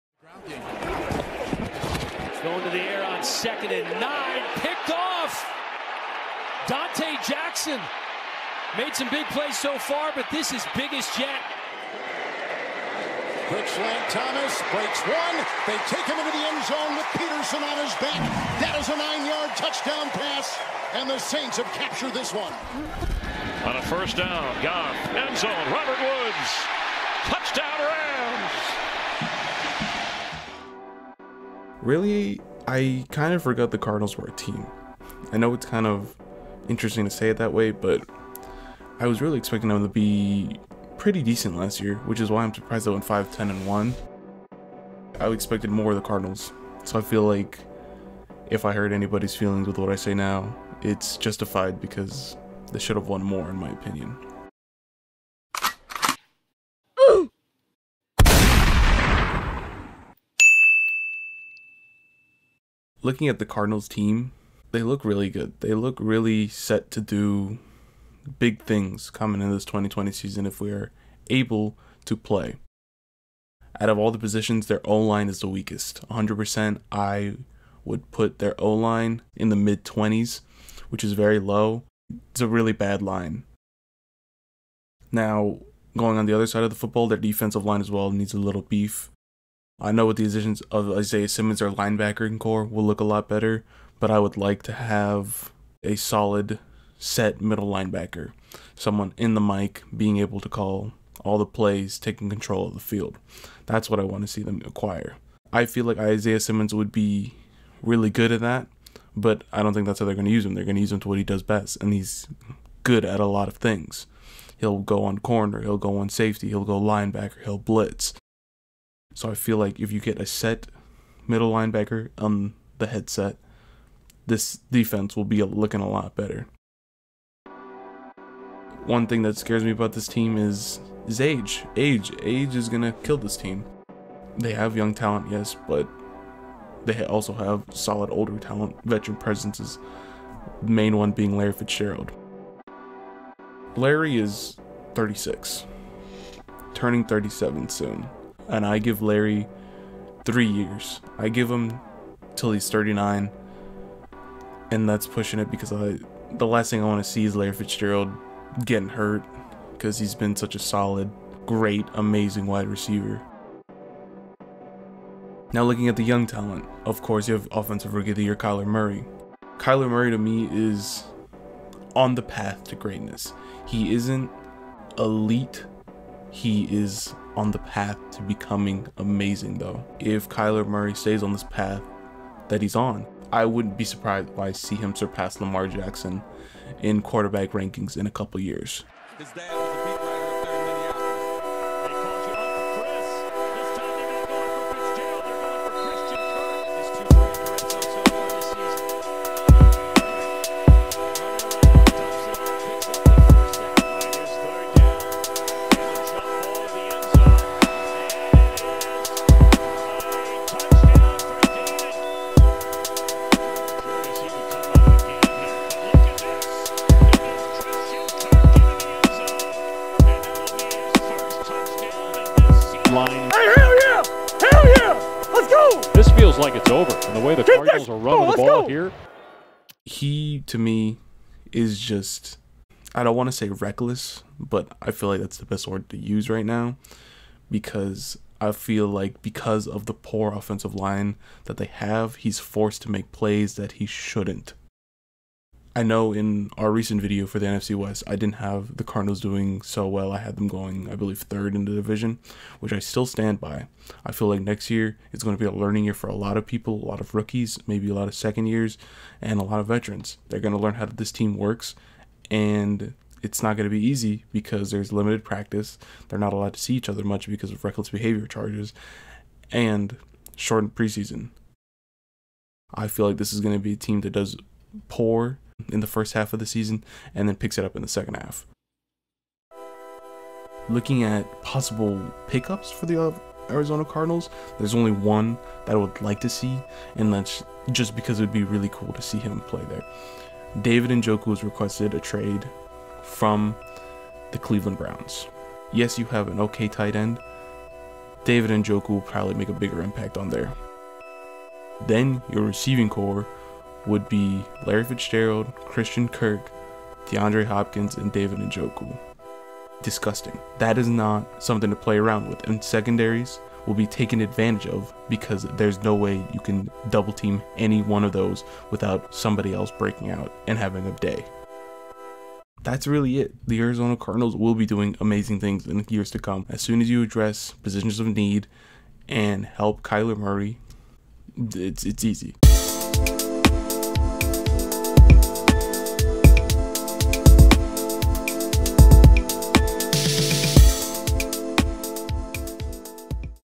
Fixing the Arizona Cardinals
In this video, Xavier discusses what the Cardinals already have and what they need to obtain in order to become a contender in the NFC.